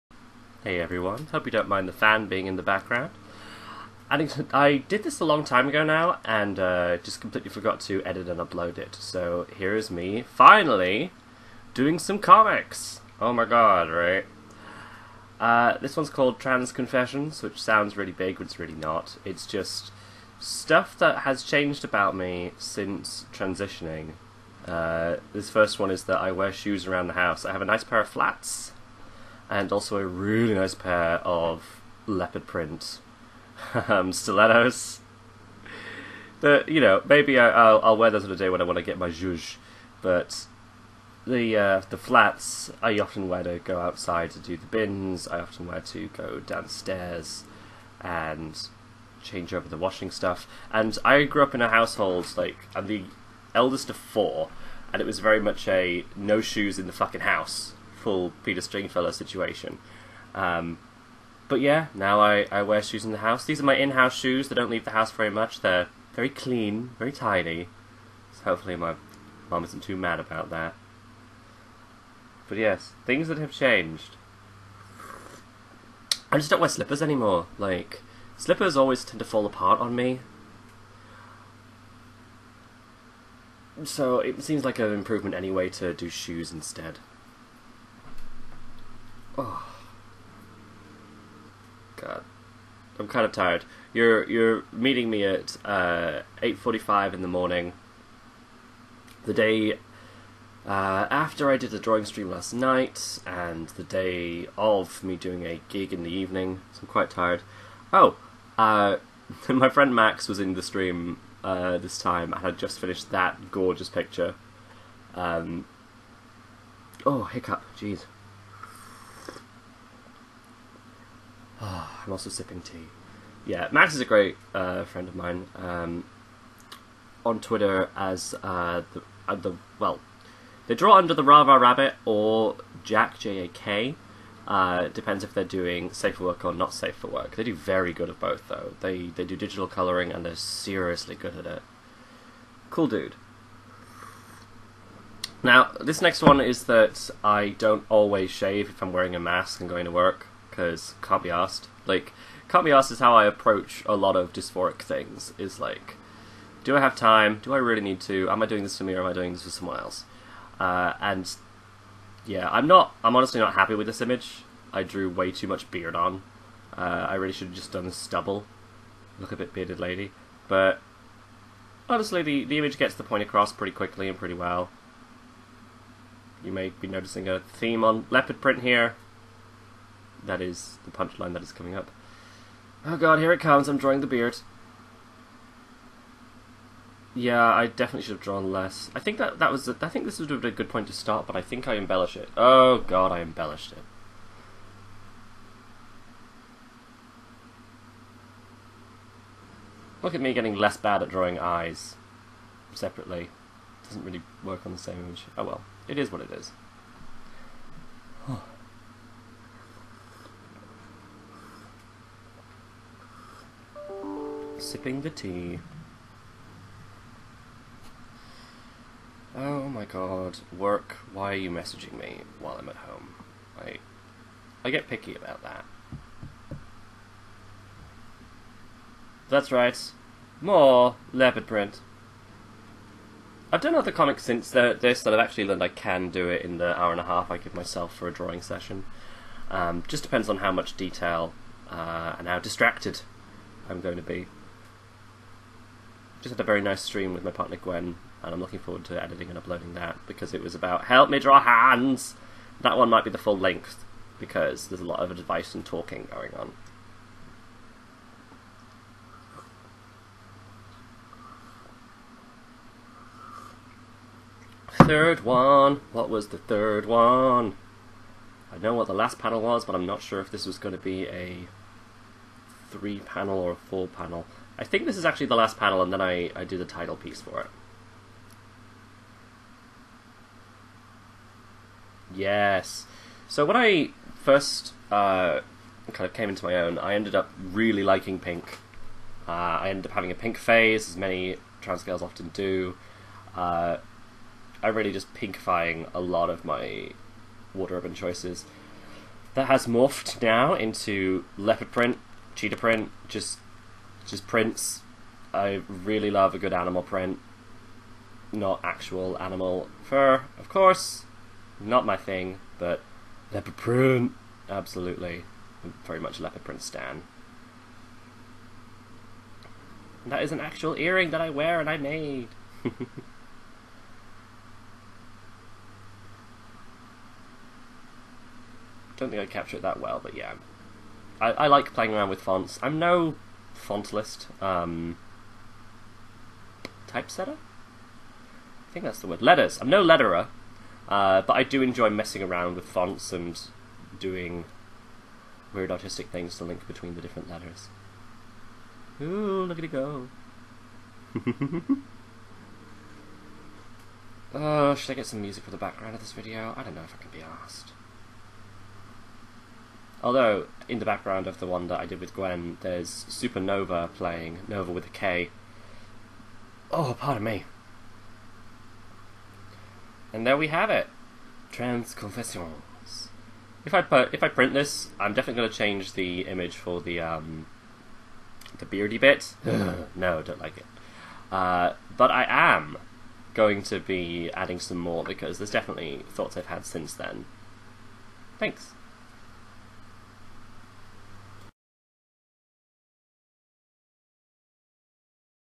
Comic Timelapse - Trans Confessions
Not even sure when I streamed this, but I finally got round to editing it!
Livestreaming Mondays from 7:30, UK Time.

And shout-out to my friend Maxwell, whose an excellent digital illustrator [Heart]